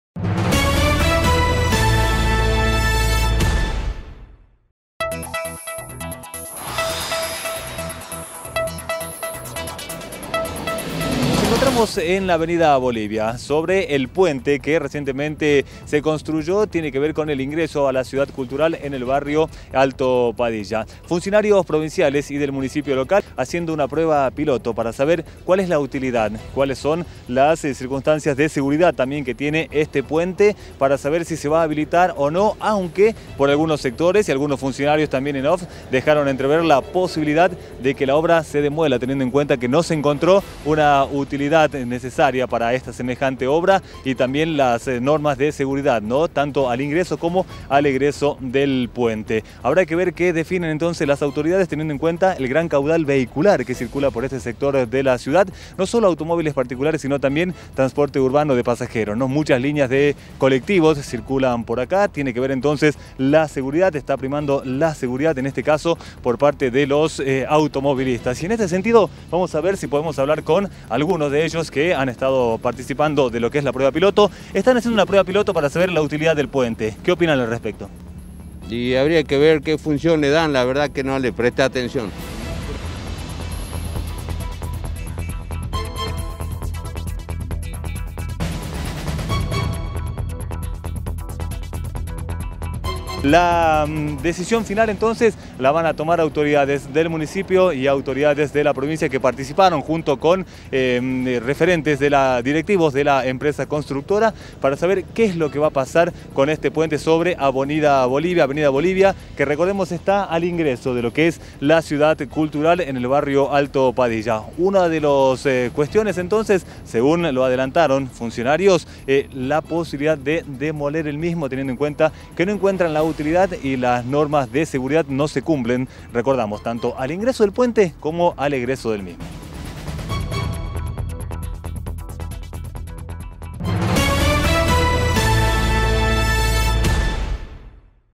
“Los puentes de NadieSon”
Parafraseando una taquillera película de Clint Eastwood, “Los puentes de Madison” donde estos son sinónimo de romanticismo, en Jujuy se analiza la utilidad de un puente que aún no fue habilitado.